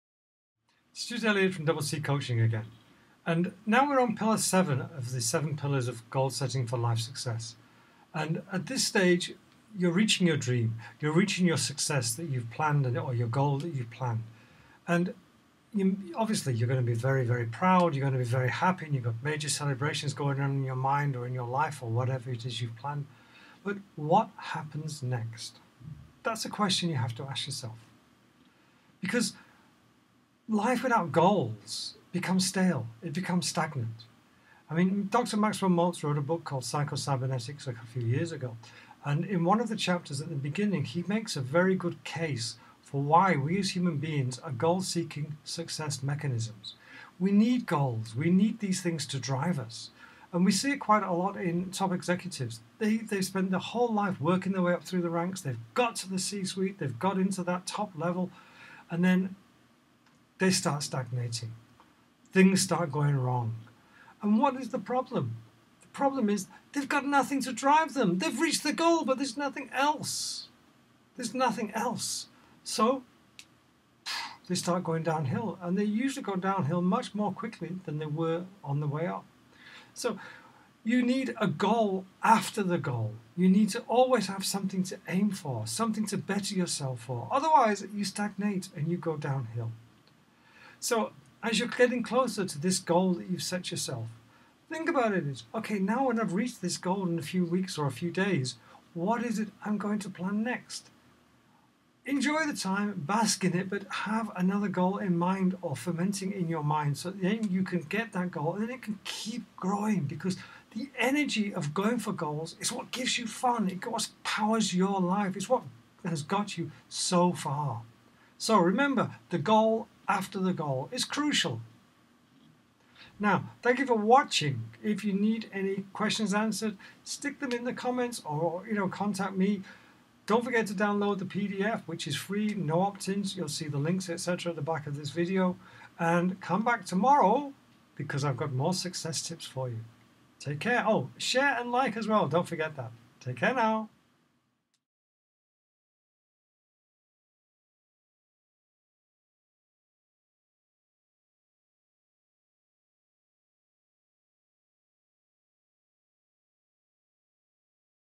You've Reached Your Goals - Whats Next? Pillar7
- If you're close to reaching your goals and think you can step off the gas a little maybe it's time to think again.